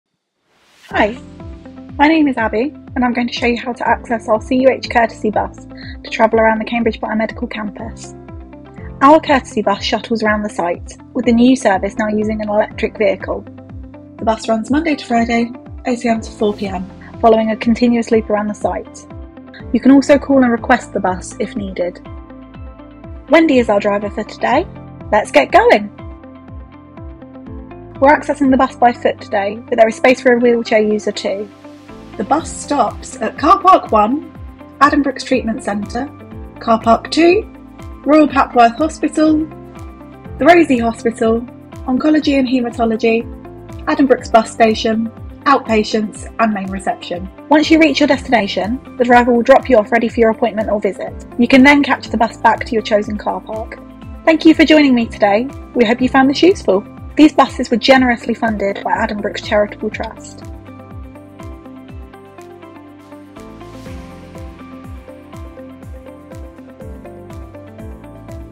Using the CUH courtesy bus - a video guide
The patient courtesy bus shuttles around site, with the service now provided using an electric vehicle.

The bus runs Monday to Friday, 08.00am- 05.00pm, following a continuous loop around the site.

We cannot offer medical advice on this, or other, social media channels. if it's not an emergency.